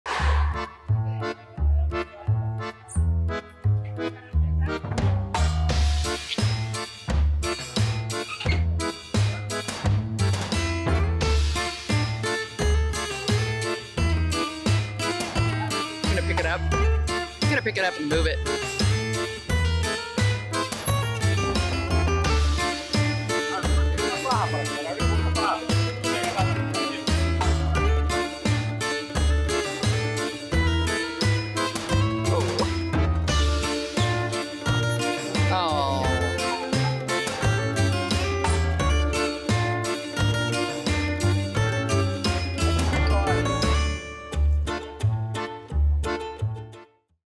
Three -Toed Sloth Crosses Road in Costa Rica | Shorts
At least he wasn't speeding... 🦥